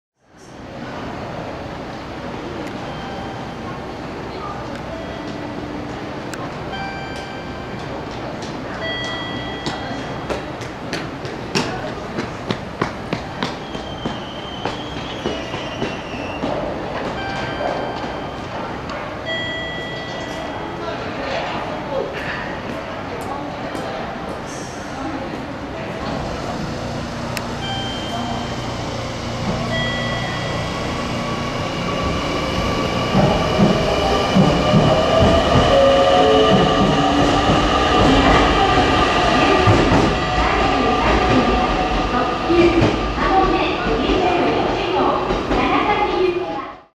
【JR九州】817系V028編成通肥前山口行＠佐賀('19/03)
JR九州長崎本線普通肥前山口行に充当される817系V028編成を佐賀駅で撮影した記録です。
JR-Kyushu ; Kyushu Railway Company
Saga Station
Saga City Saga Pref. Japan
No.11196